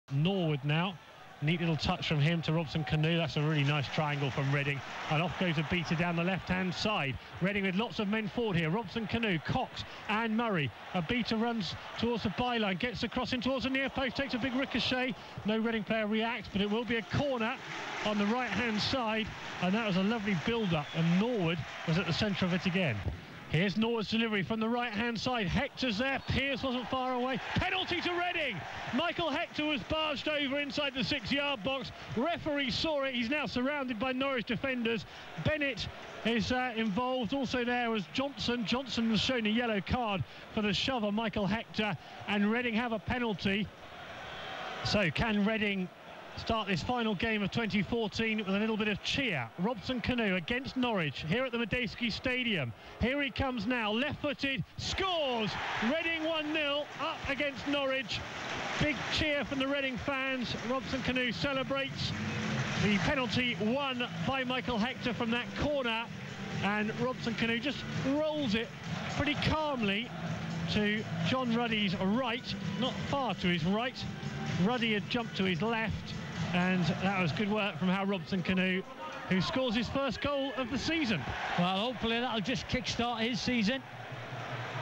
Robson-Kanu scores | Reading 2-1 Norwich | 28.12.14
Hal Robson-Kanu opens the scoring against Norwich after Michael Hector is fouled in the area.